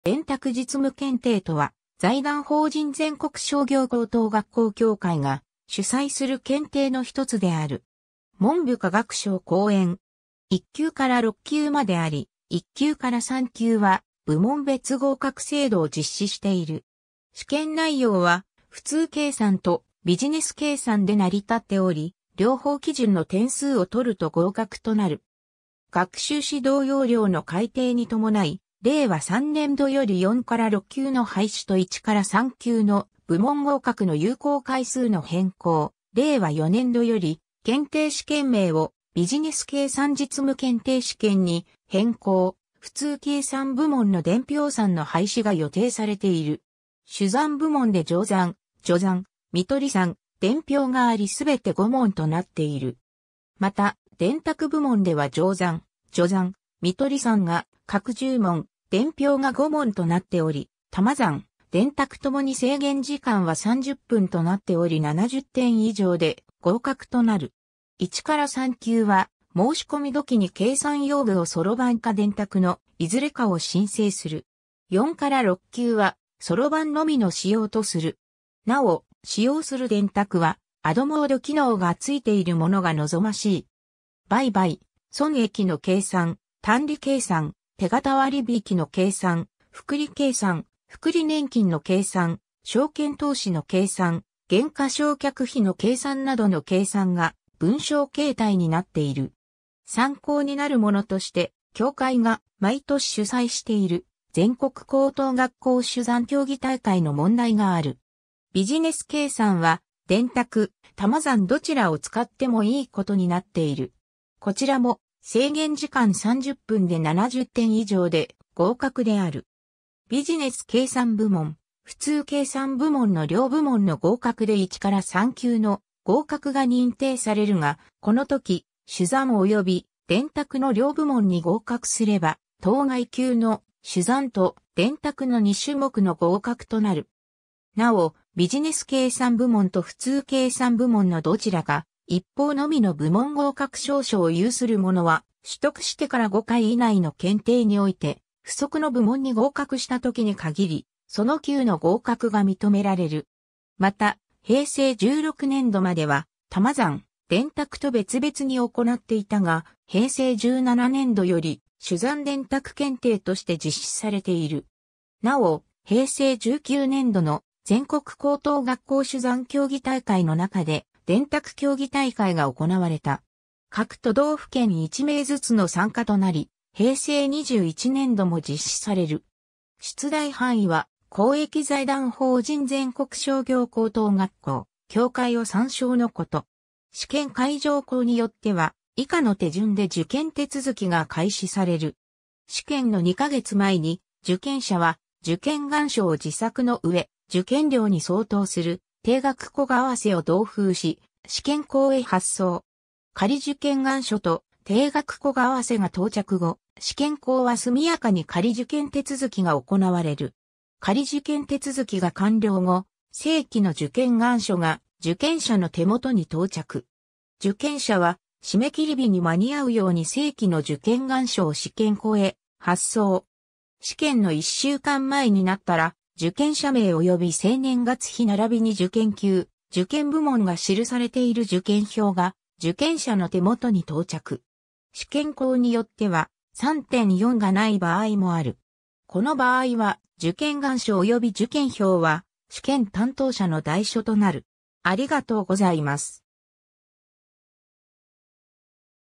全商珠算・電卓実務検定
全商珠算・電卓実務検定（ぜんしょうしゅざん・でんたくじつむけんてい）とは、財団法人全国商業高等学校協会が主催する検定のひとつである。
文部科学省後援。
 1級から6級まであり、1級～3級は部門別合格制度を実施している。
 試験内容は「普通計算」と「ビジネス計算」で成り立っており、両方基準の点数を取ると合格となる。
　 学習指導要領の改訂に伴い、令和3年度より4～6級の廃止と1～3級の部門合格の有効回数の変更(5回→4回)、令和4年度より検定試験名を「ビジネス計算実務検定試験」に変更、普通計算部門の伝票算の廃止が予定されている。
 珠算部門で乗算・除算・見取算・伝票がありすべて5問となっている。
また、電卓部門では乗算・除算・見取算が各10問、伝票が5問となっており珠算・電卓ともに制限時間は30分となっており70点以上で合格となる。
 1～ 3級は申込時に計算用具をそろばんか電卓のいずれかを申請する。
4～6級は「そろばん」のみの使用とする。
 なお、使用する電卓はアドモード (ADD2) 機能がついているものが望ましい。
 売買・損益の計算、単利計算、手形割引の計算、複利計算、複利年金の計算、証券投資の計算、減価償却費の計算などの計算が文章形態になっている。
 参考になるものとして、協会が毎年主催している全国高等学校珠算競技大会の問題がある。
ビジネス計算は電卓・珠算どちらを使ってもいいことになっている。
こちらも制限時間30分で70点以上で合格である。
 ビジネス計算部門、普通計算部門の両部門の合格で1～3級の合格が認定されるが、このとき「珠算」および「電卓」の両部門に合格すれば，当該級の「珠算」と「電卓」の２種目の合格となる。
なお、ビジネス計算部門と普通計算部門のどちらか一方のみの部門合格証書を有する者は，取得してから 5 回以内の検定において不足の部門に合格したときに限り，その級の合格が認められる。
 また、平成16年度までは珠算・電卓と別々に行っていたが、平成17年度より珠算電卓検定として実施されている。
なお、平成19年度の全国高等学校珠算競技大会の中で、電卓競技大会が行われた。
各都道府県1名ずつの参加となり平成21年度も実施される。
 出題範囲は、公益財団法人全国商業高等学校協会を参照のこと。
 試験会場校（以下、試験校と略す）によっては、以下の手順で受験手続きが開始される。
 試験の2ヶ月前に、受験者は受験願書を自作の上、受験料に相当する定額小為替を同封し、試験校へ発送。
 仮受験願書と定額小為替が到着後、試験校は速やかに仮受験手続きが行われる。
 仮受験手続きが完了後、正規の受験願書が受験者の手元に到着。
 受験者は、締切日に間に合うように正規の受験願書を試験校へ発送。
 試験の1週間前になったら、受験者名及び生年月日並びに受験級・受験部門が記されている受験票が、受験者の手元に到着（受験票は、試験担当者が代書）。
 試験校によっては、3・4がない場合もある。
この場合は、受験願書及び受験票は、試験担当者の代書となる。